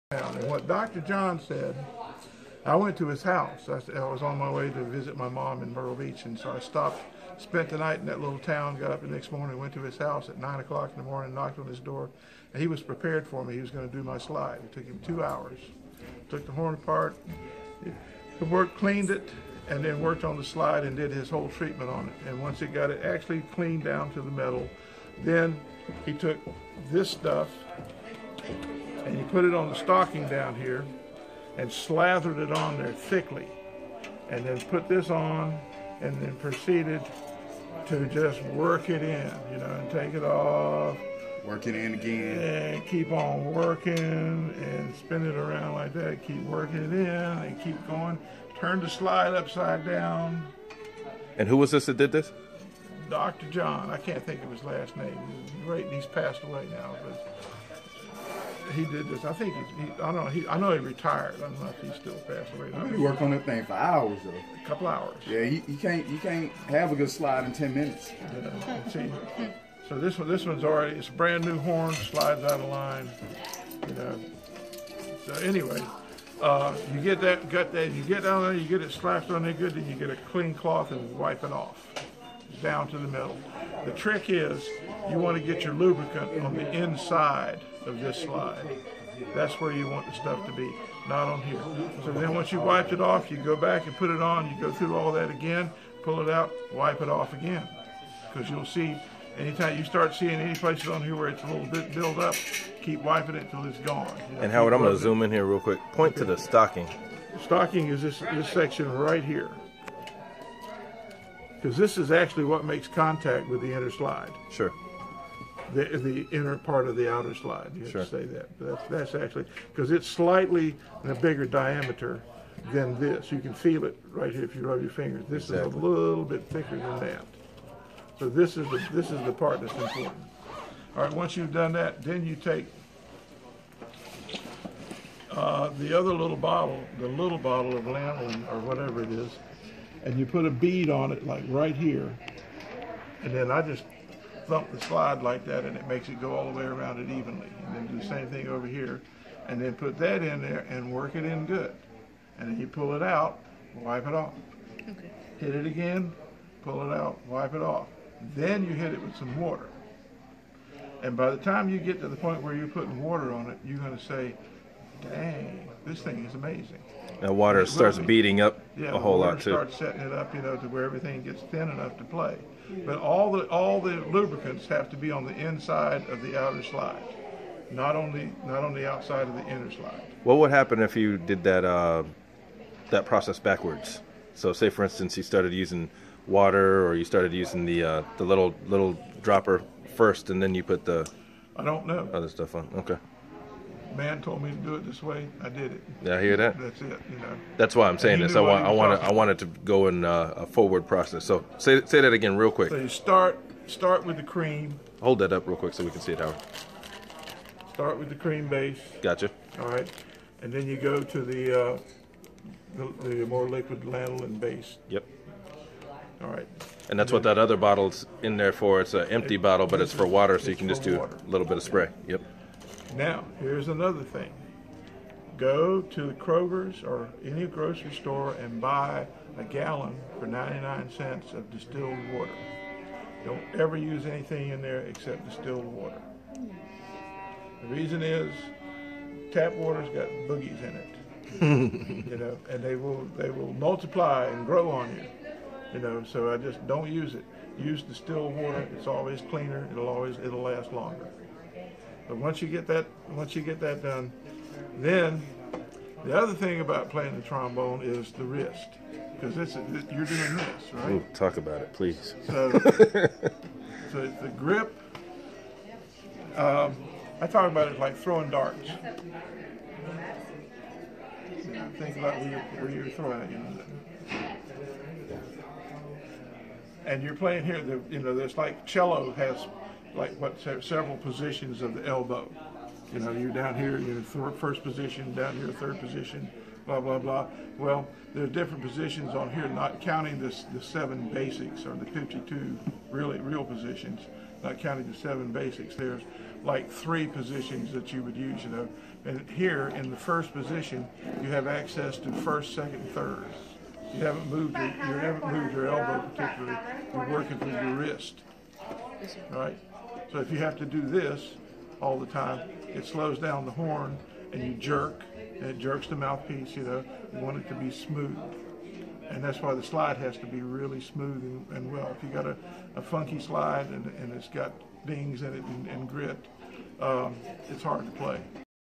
Trombone - Trombone Slide Products and Care - Howard Lamb
I would like to thank Howard Lamb (Band Leader of The Memphis Jazz Orchestra) for taking time to teach and inspire the trombonists of the future. Howard did a great job discussing:
- Slide Care and Maintenance
- Hand Position
- Wrist Flexibility
- Products and practices to avoid
- Slide Anatomy